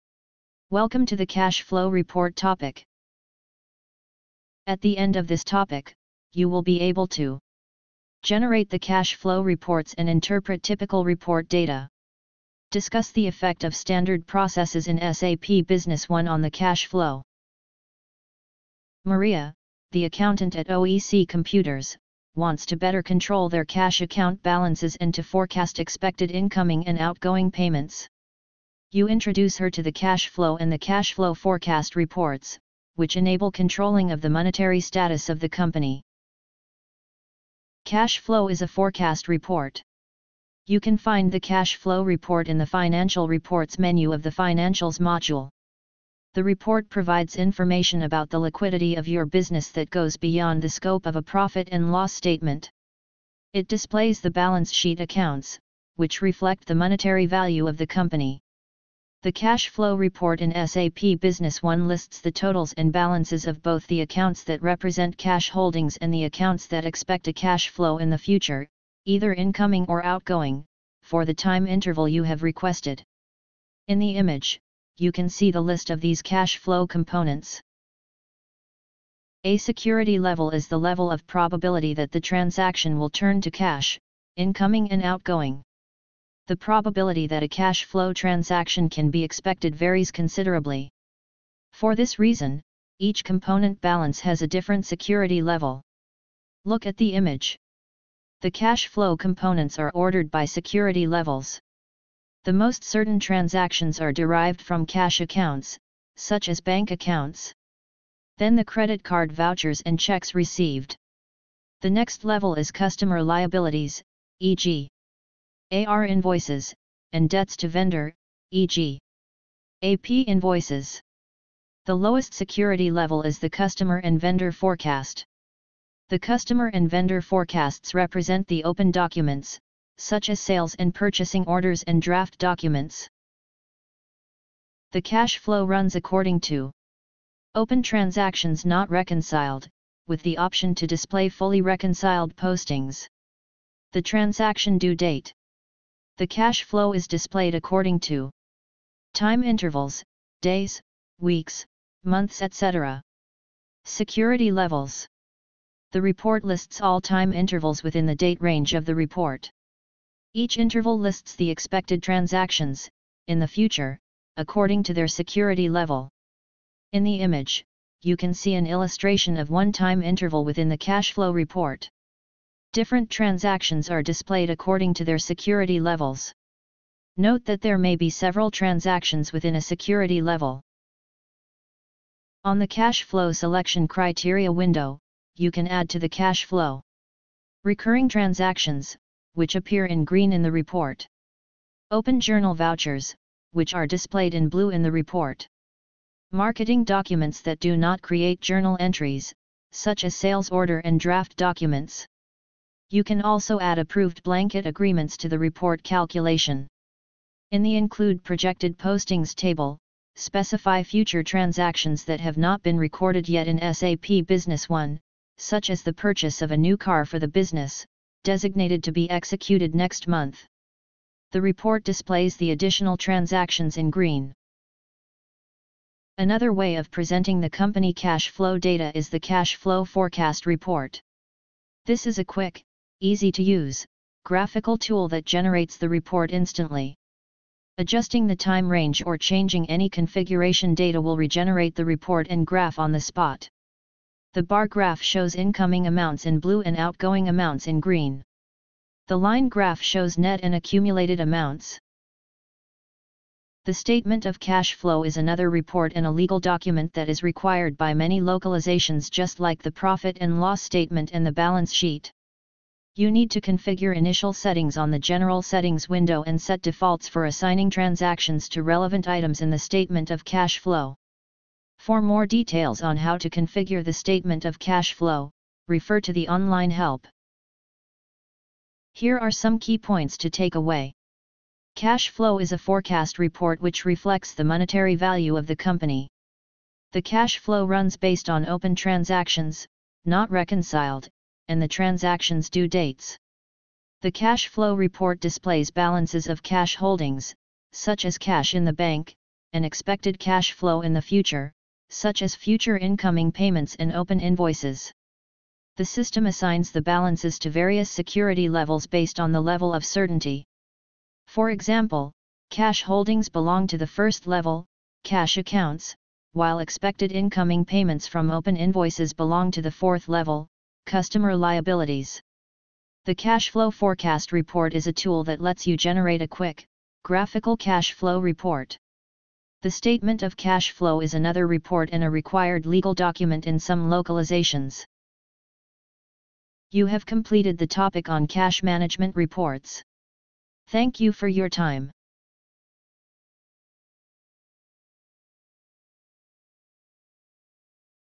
TB1100-05-22 SAP Business One 9.3 - Cash Flow Report
Welcome to the Cash Flow report topic.

At the end of this topic, you will be able to:
- Generate the Cash Flow reports and interpret typical report data.
- Discuss the effect of standard processes in SAP Business One on the Cash Flow

Maria, the accountant at OEC computers, wants to better control their cash account balances and to forecast expected incoming and outgoing payments.
You introduce her to the Cash Flow and the Cashflow Forecast reports, which enable controlling of the monetary status of the company.

Cash flow is a forecast report –
You can find the Cash Flow report in the Financial Reports menu of the Financials module.
The report provides information about the liquidity of your business that goes beyond the scope of a profit and loss statement.
It displays the balance sheet accounts, which reflect the monetary value of the company.
The cash flow report in SAP Business One lists the totals and balances of both the accounts that represent cash holdings and the accounts that expect a cash flow in the future (either incoming or outgoing) for the time interval you have requested. In the image, you can see the list of these cash flow components.

A security level is the level of probability that the transaction will turn to cash (incoming and outgoing). The probability that a cash flow transaction can be expected varies considerably. For this reason, each component balance has a different security level.
- Look at the image. The cash flow components are ordered by security levels:
  - The most certain transactions are derived from cash accounts, such as bank accounts
  - Then the credit card vouchers and checks received
  - The next level is Customer Liabilities (e.g. AR Invoices) and Debts to Vendor (e.g. AP Invoices)
  - The lowest security level is the customer and vendor forecast. The customer and vendor forecasts represent the open documents, such as sales and purchasing orders and draft documents.

The Cash Flow runs according to:
- Open transactions – not reconciled (with the option to display fully reconciled postings).
- The transaction Due Date.
The Cash Flow is displayed according to:
- Time Intervals (days, weeks, months etc.)
- Security levels
The report lists all time intervals within the date range of the report. Each interval lists the expected transactions (in the future) according to their security level.
In the image, you can see an illustration of one time interval within the cashflow report.
Different transactions are displayed according to their security levels.
Note that there may be several transactions within a security level.

On the Cash Flow Selection Criteria window, you can add to the cash flow:
- Recurring transactions, which appear in green in the report
- Open journal vouchers, which are displayed in blue in the report
- Marketing documents that do not create journal entries, such as sales order and draft documents.
- You can also add approved blanket agreements to the report calculation.
In the Include Projected Postings table, specify future transactions that have not been recorded yet in SAP Business One, such as the purchase of a new car for the business, designated to be executed next month. The report displays the additional transactions in green.

Another way of presenting the company cash flow data is the Cash Flow Forecast report. This is a quick, easy to use, graphical tool that generates the report instantly.
Adjusting the time range or changing any configuration data will regenerate the report and graph on the spot.
The bar graph shows incoming amounts in blue and outgoing amounts in green. The line graph shows net and accumulated amounts.

The Statement of Cash Flow is another report and a legal document that is required by many localizations – just like the profit and loss statement and the balance sheet.
You need to configure initial settings on the General Settings window and set defaults for assigning transactions to relevant items in the Statement of Cash Flow.
For more details on how to configure the Statement of Cash Flow, refer to the online help.